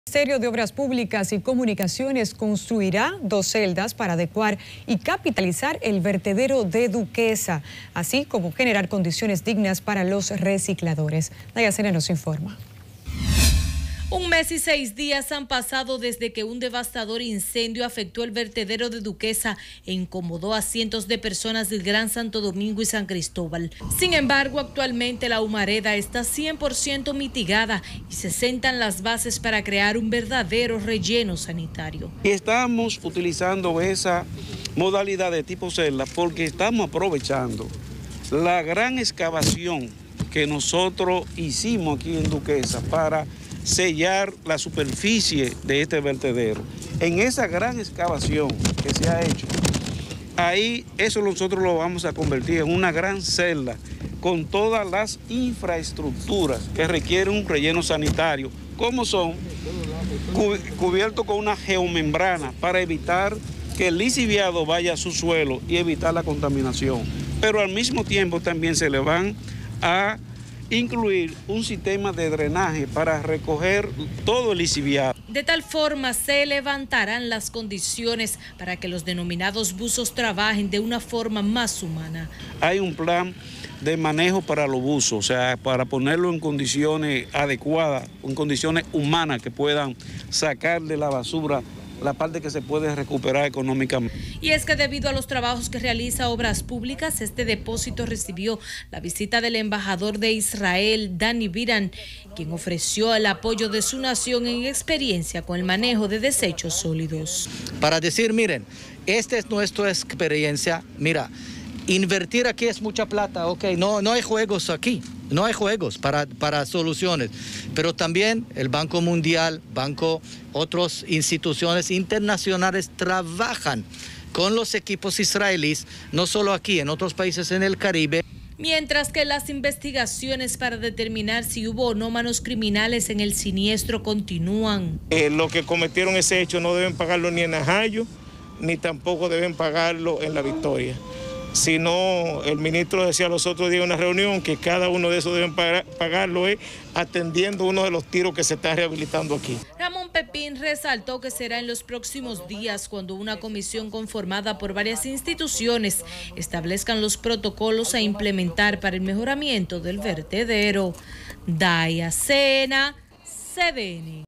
Crearán celdas para adecuar relleno Duquesa; embajador Israel ofrece apoyo
El Ministerio de Obras Públicas  y Comunicaciones construirá dos celdas para adecuar y capitalizar el vertedero de Duquesa, así como generar condiciones dignas para los recicladores.

Cuestionado sobre a cuánto podrían elevarse el presupuesto de las elecciones  por la implementación del protocolo sanitario y nuevas medidas de seguridad  para la celebración de las elecciones del 05 de julio, Castaños Guzmán, señaló que habría que hacer cálculos para precisar una suma exacta.

Un mes y seis días han pasado desde que un devastador incendio afectó el vertedero de Duquesa e incomodó a cientos de personas del Gran Santo Domingo y San Cristóbal, sin embargo, actualmente la humareda está 100 por ciento mitigada y se sentan las bases para crear un verdadero relleno sanitario.

 De tal forma se crearán las condiciones para que los denominados "buzos" trabajen de una forma más humana.